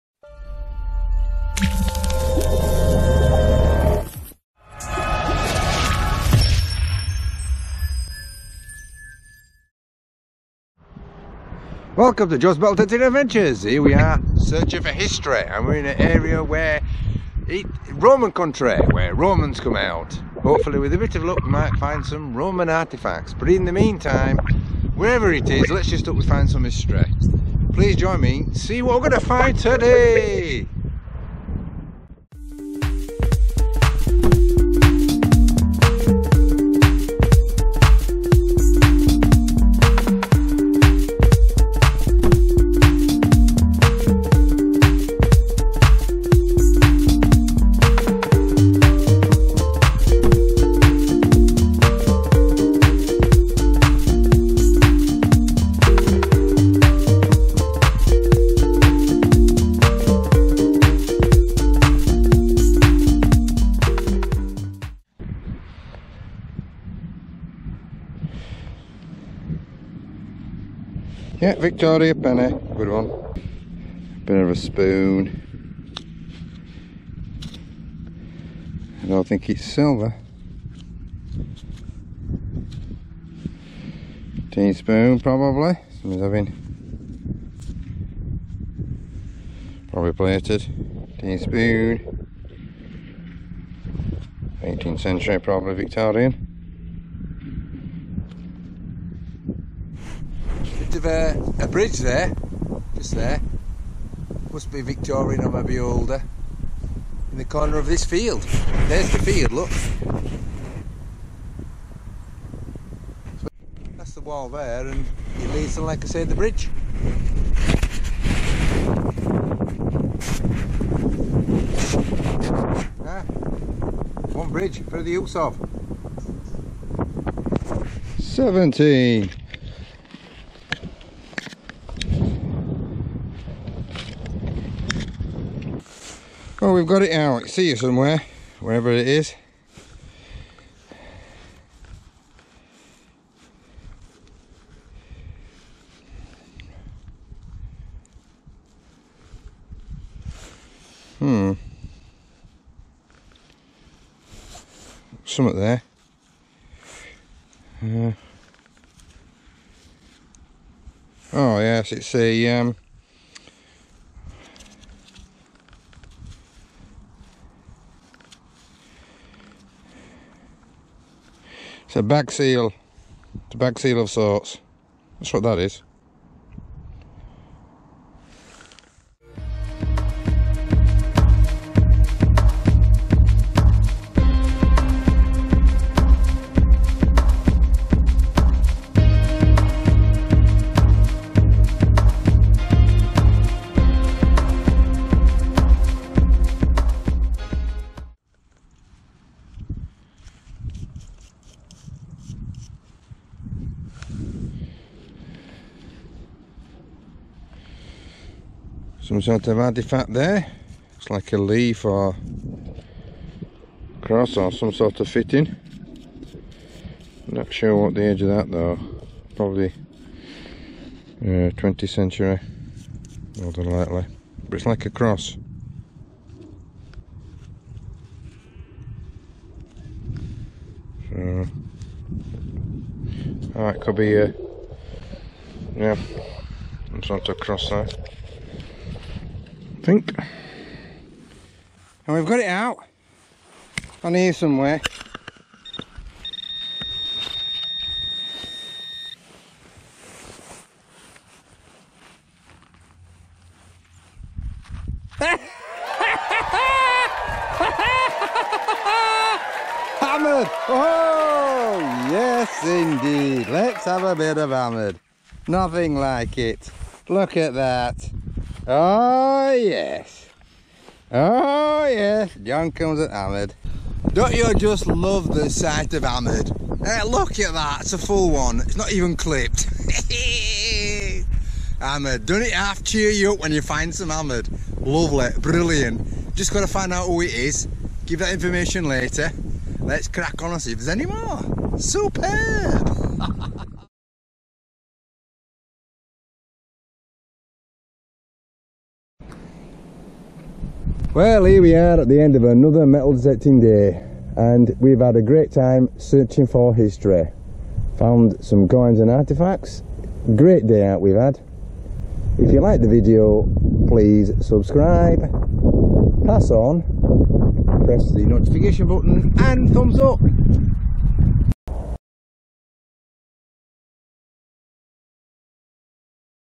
MEDIEVAL SILVER HAMMERED COIN, BUT YOU IS THE KING & WHICH CENTURY IS IT & DID I FIND MORE HISTORY?
I am metal detecting in a ancient town here the roman and medieval people were. i found a medieval silver hammered coin and many other coins and artefacts.  I also try to capture nature's seasons, animals I encounter and insects activity.